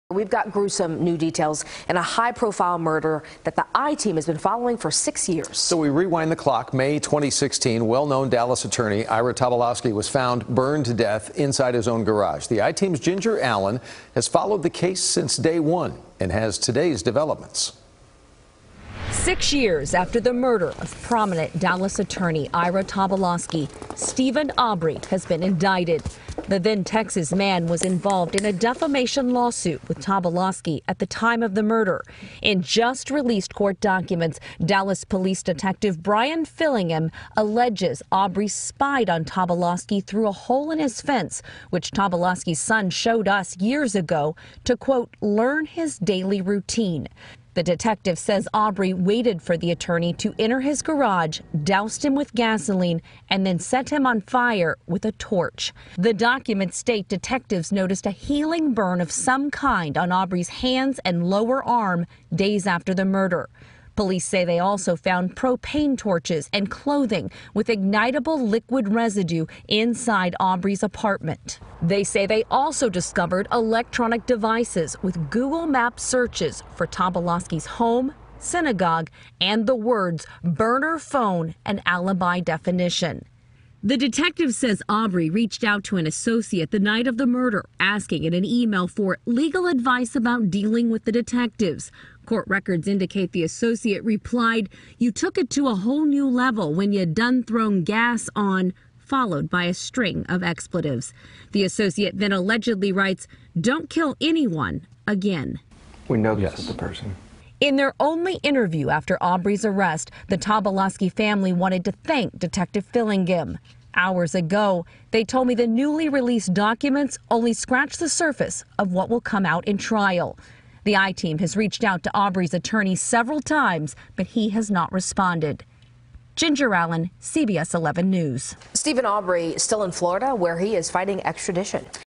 I-Team Update: Gruesome details emerge in newly-released documents in Ira Tobolowsky murder
Gruesome new details have emerged in the high-profile murder of Dallas attorney Ira Tobolowsky.